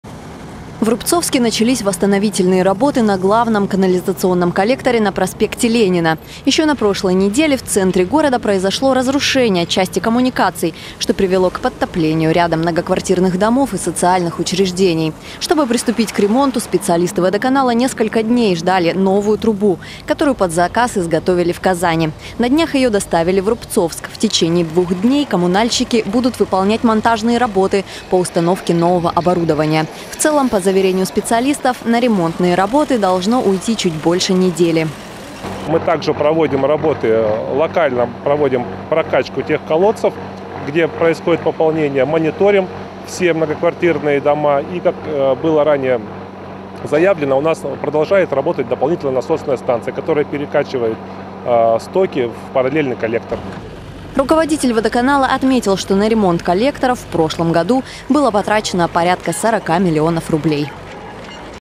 Ремонт коллектора в Рубцовске завершат на следующей неделе
В течение двух дней коммунальщики будут выполнять монтажные работы по установке нового оборудования. В целом, по заверению специалистов, на ремонтные работы должно уйти чуть более недели.